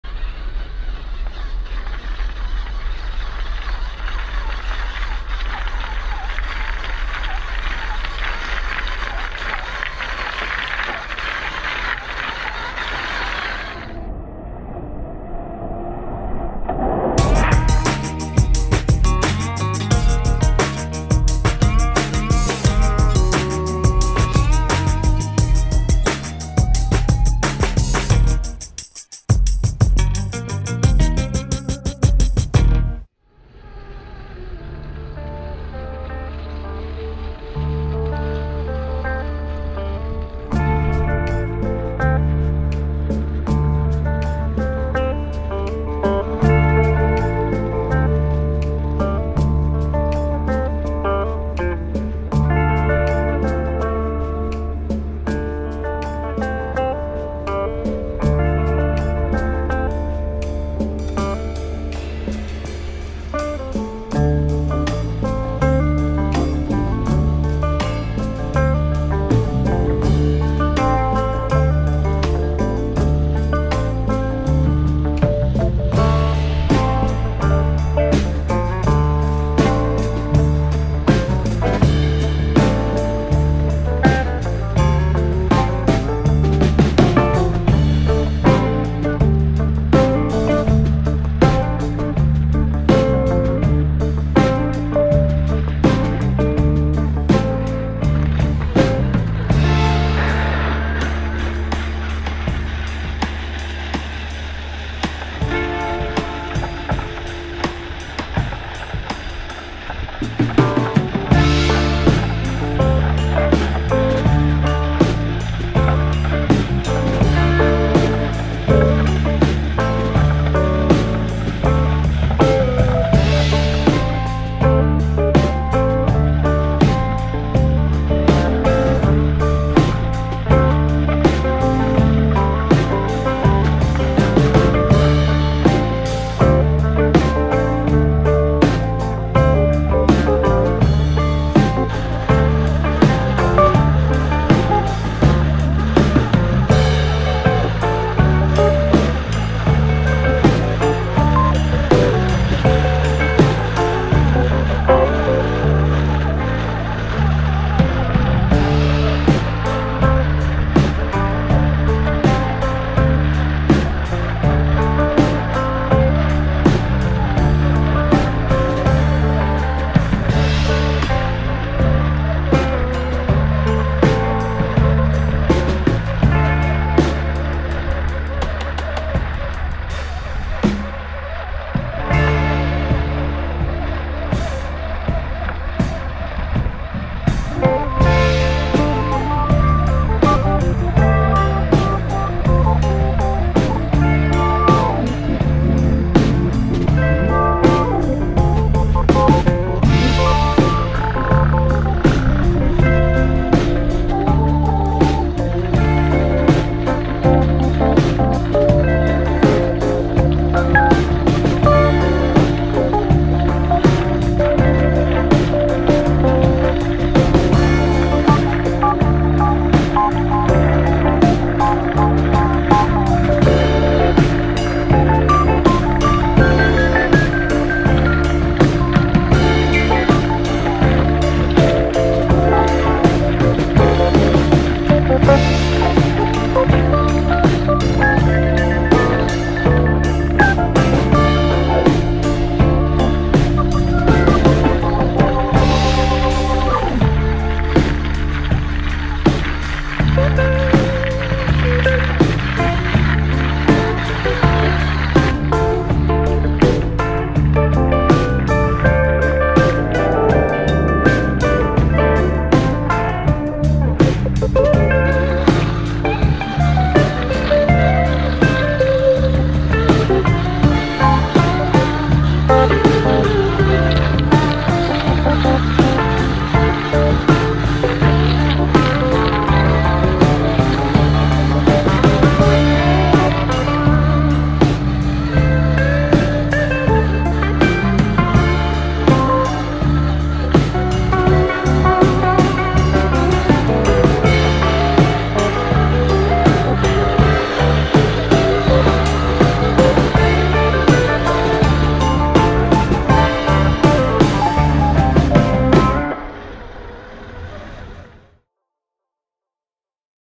Axial SCX10 III Early Ford Bronco - Rock Pile
Today I filmed my Axial SCX10 III running on a rock pile. I have lowered this truck as the stock suspension made it sit too high. I have done my own decal set and swapped over to some Nitto Trail Grappler tires that I painted in the white lettering. I also silver painted part of the front grill and all the shifter knobs.